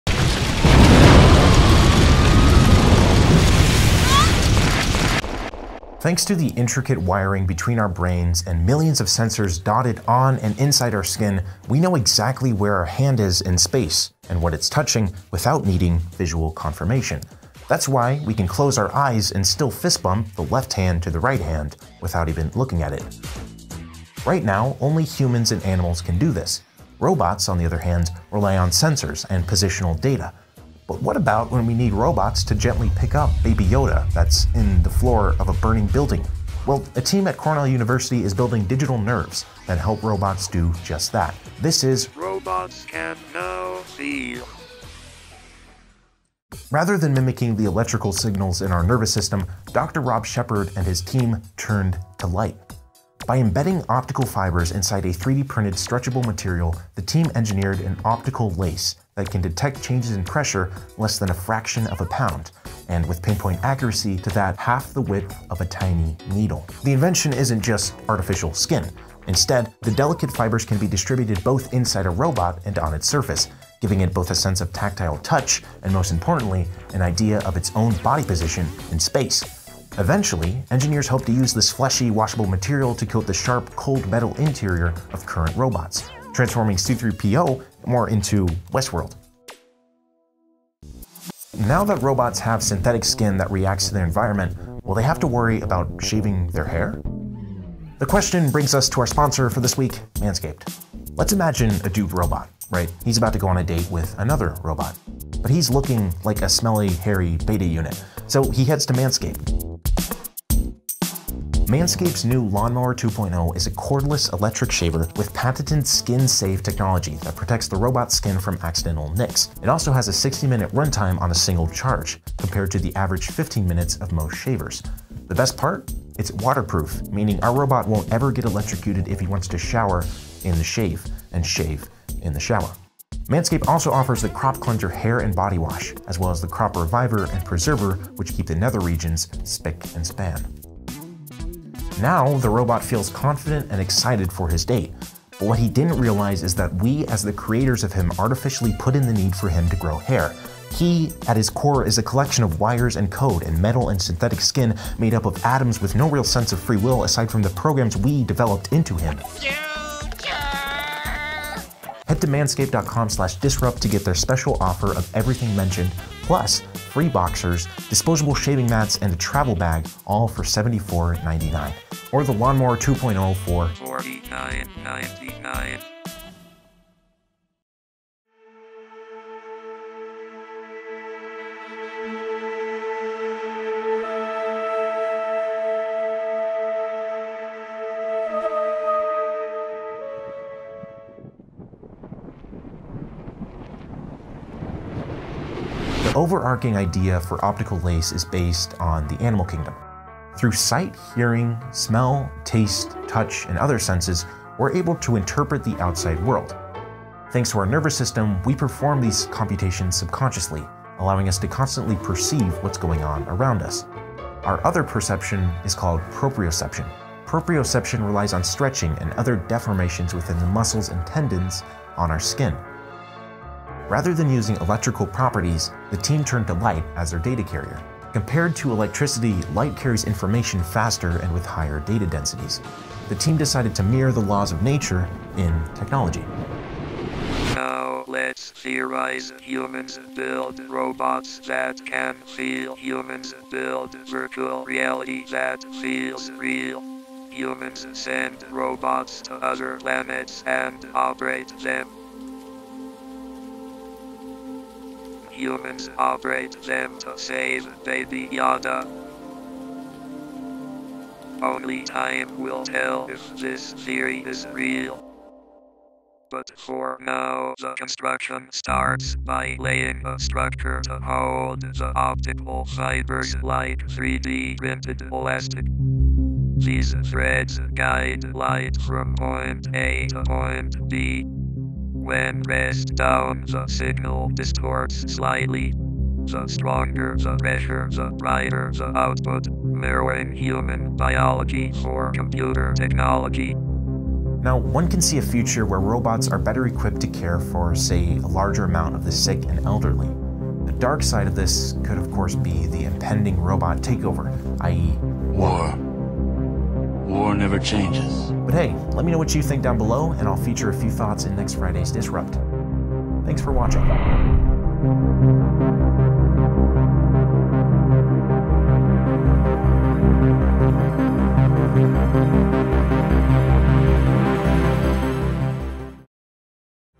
Robots Can Now ░f░e░e░l░
A new technology gives robots a sense of touch and proprioception, what does this mean for the future of h u m a n k i n d?